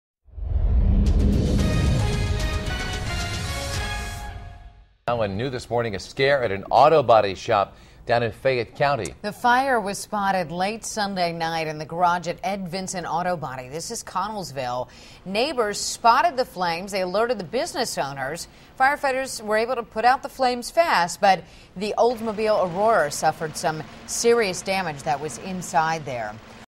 Fire Breaks Out In Connellsville Body Shop
Fire was spotted in the garage at Ed Vincent Auto Body in Connellsville.